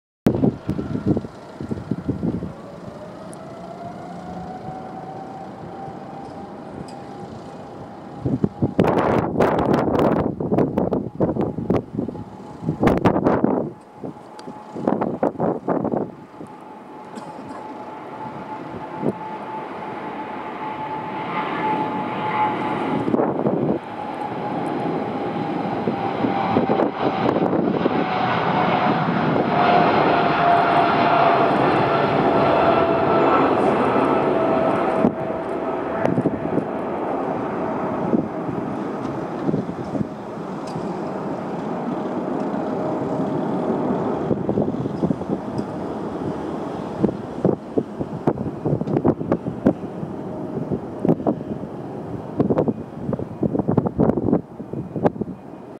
Emirates Flight 40 (BHX to Dubai)
Emirates Flight 40 from Birmingham Airport to Dubai. The aircraft is an Boeing 777-31H(ER) (reg: A6-ENA) is 3.3 years old and has been operating for Emirates since 2012.